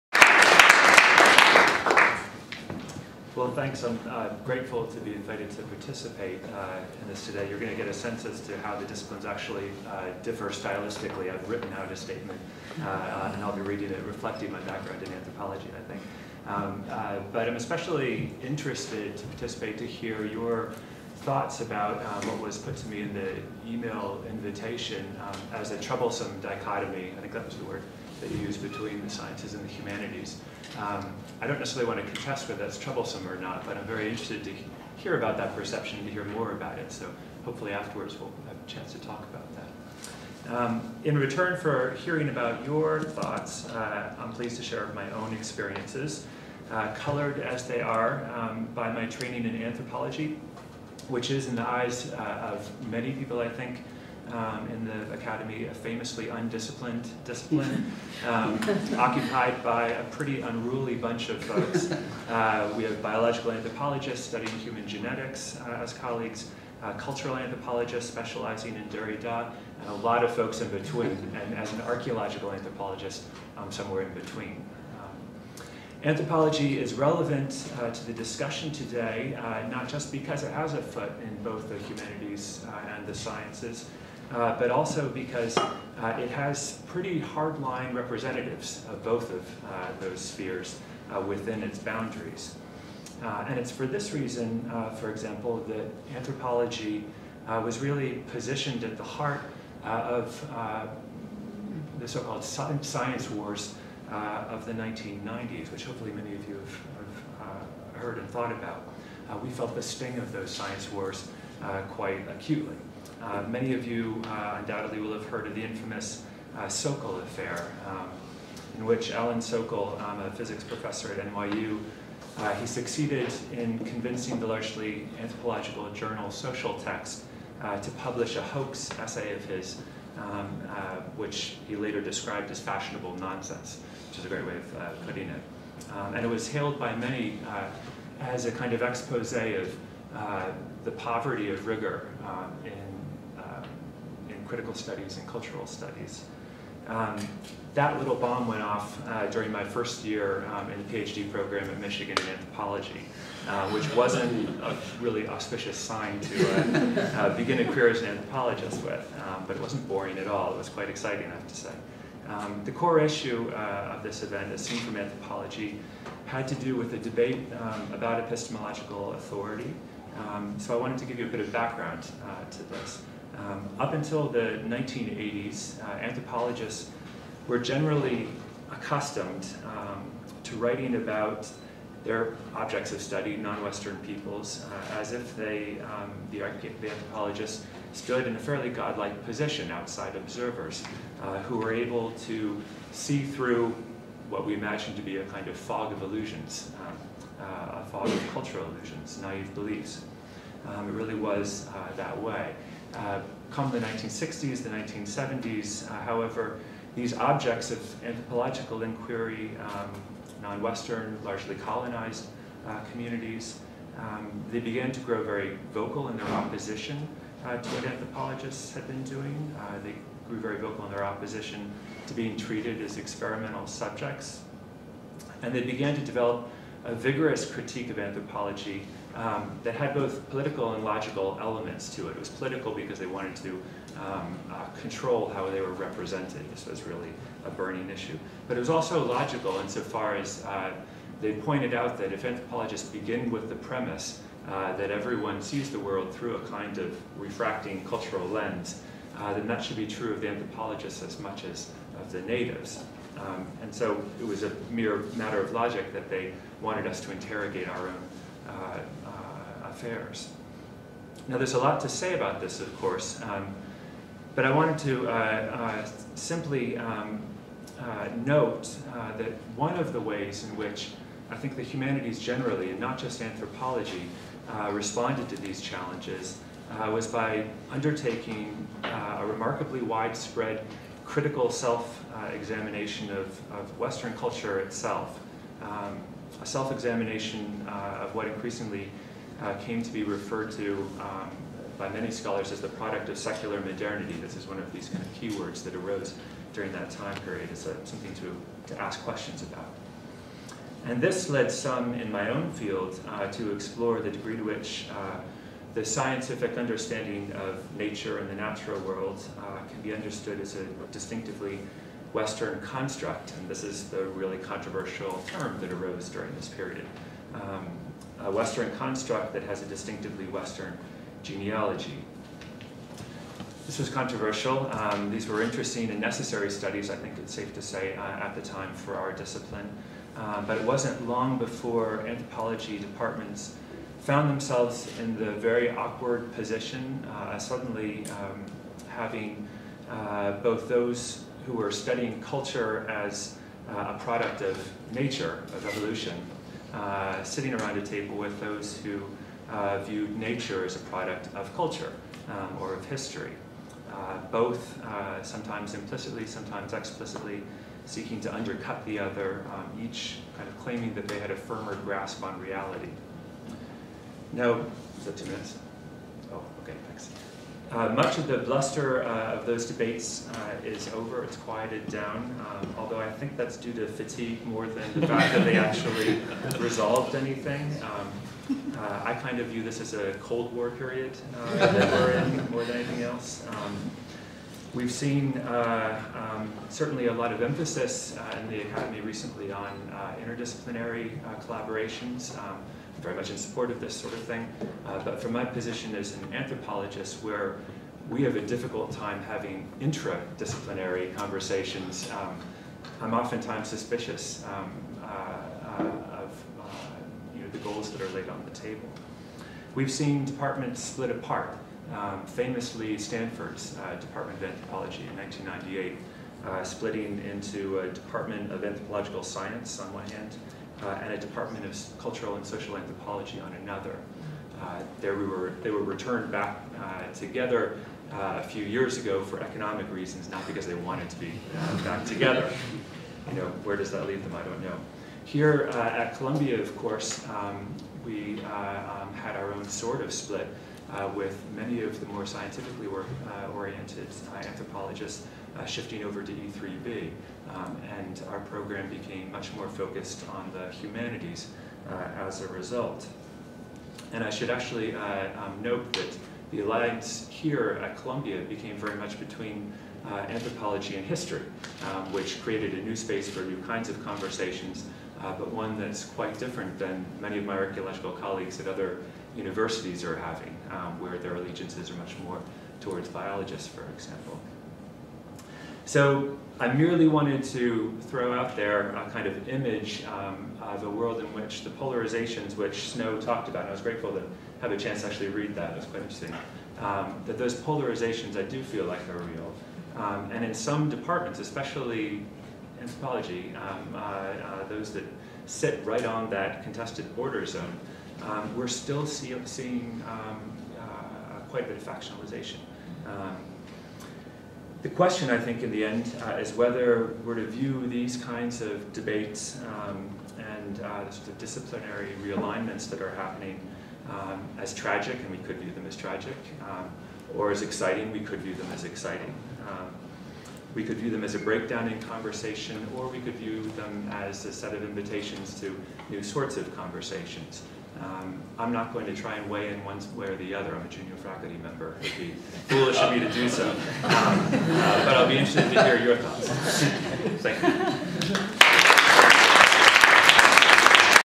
Professor Severin Fowles Speaks at SEBS Science and Humanities Panel
Science and Humanities Panel Discussion Event
Panelists: Dean Michele Moody-Adams, Professor James Valentini, Professor Philip Kitcher and, Professor Severin Fowles

Purpose: Mitigate differences between the sciences and
humanities, and raise awareness of the issue

Moderators: Sheldon Kwok and Yang Hu

Monday, April 18, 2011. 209 Havemeyer, Columbia University

Recorded by Ismael Garcia